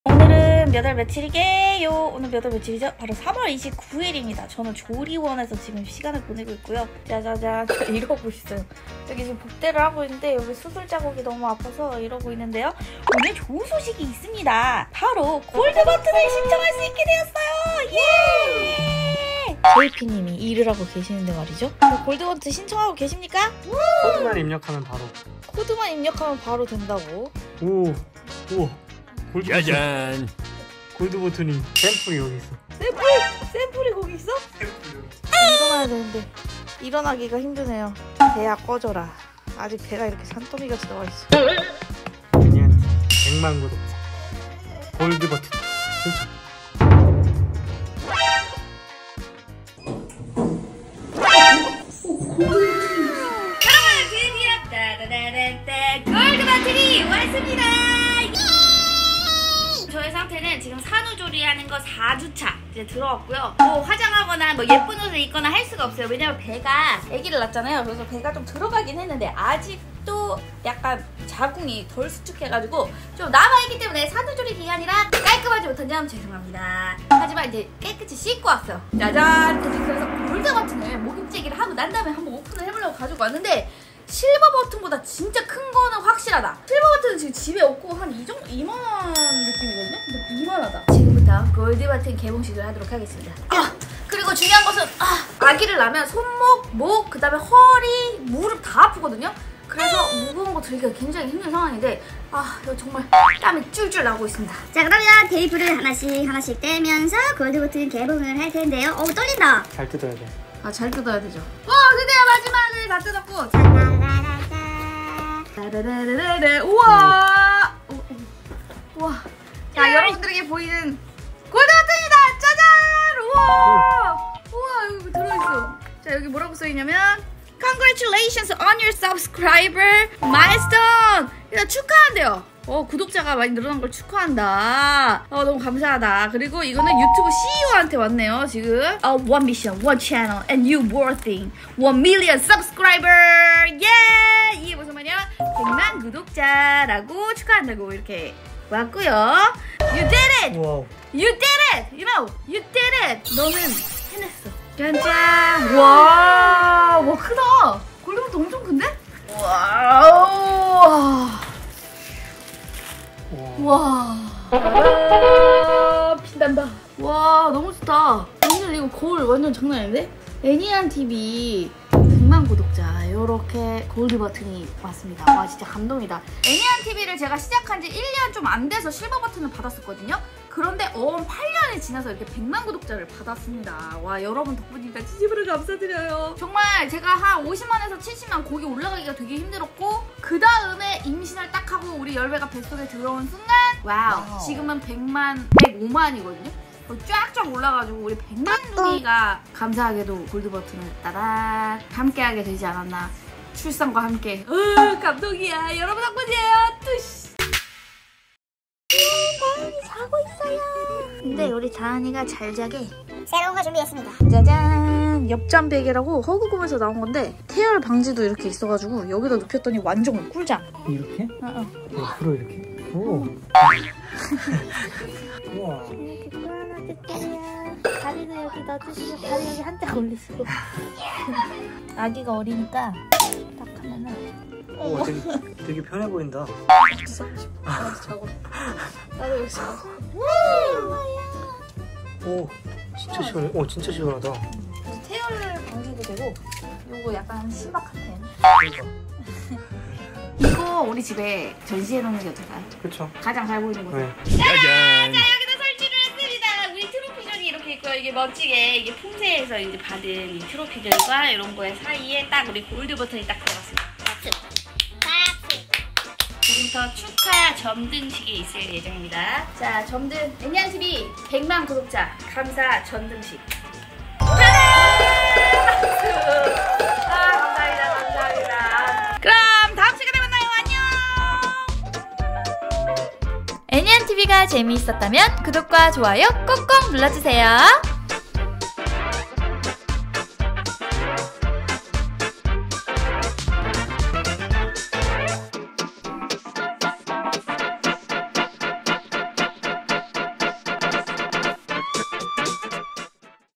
🎇드디어 골드버튼을 받았어요!!!🎇 [애니한TV]
100만 골드버튼과 함께 백만둥이의 탄생까지!
구독자분들 진심으로 감사합니다🥰